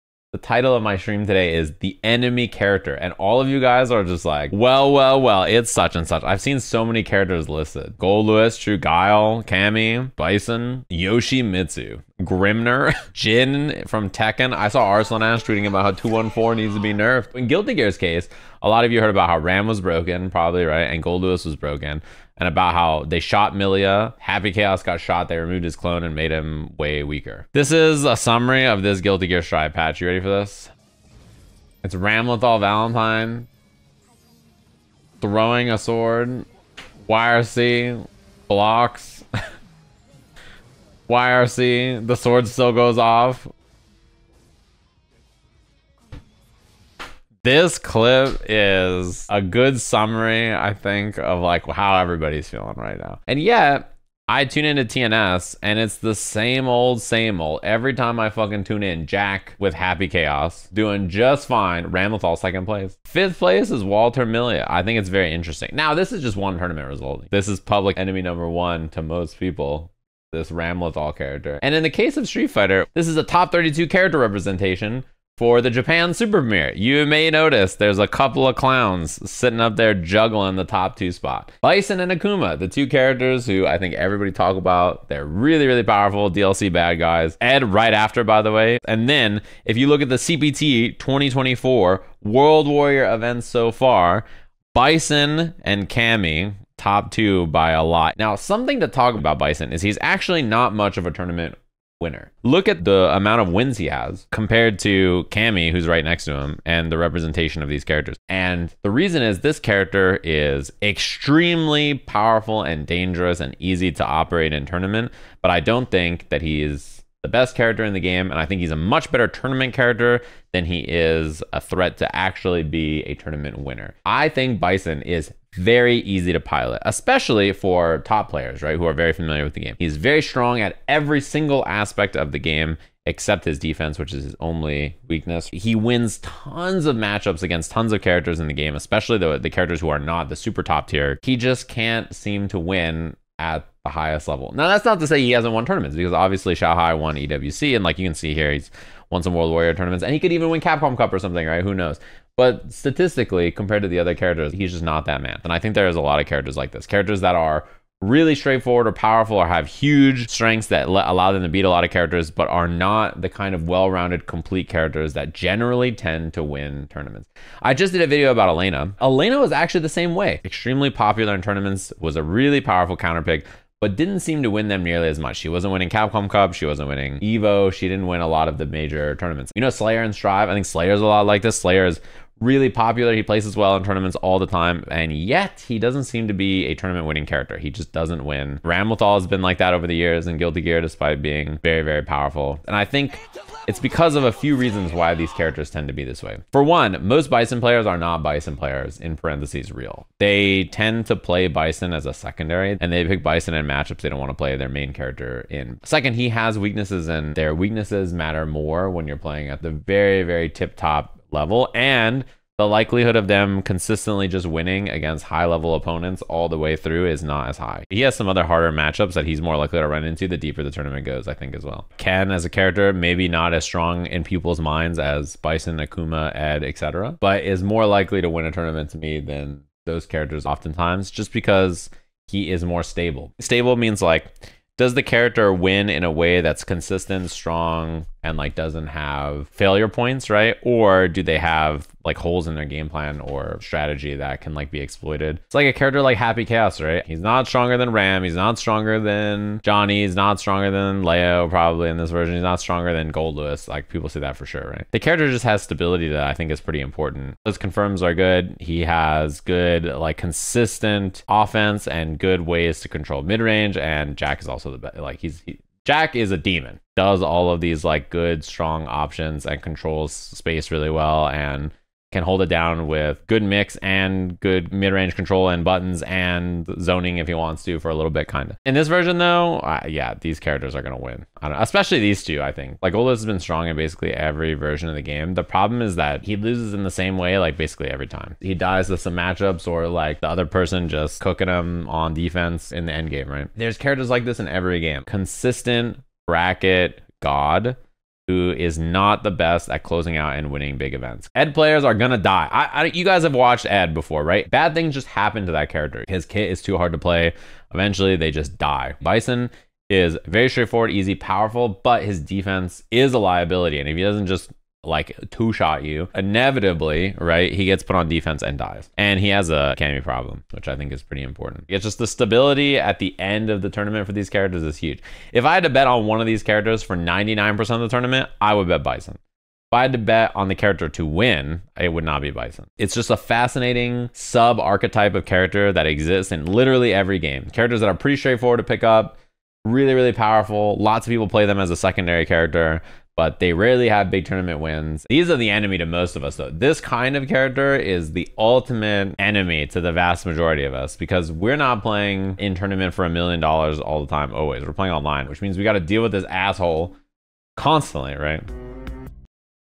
The Extremely Strong Characters that Rarely Win Tournaments
It's a very specific sub-archetype that exists in nearly every game.
streamed Nov. 5, 2024

If you're ever confused by some terminology try looking it up in the FG Glossary: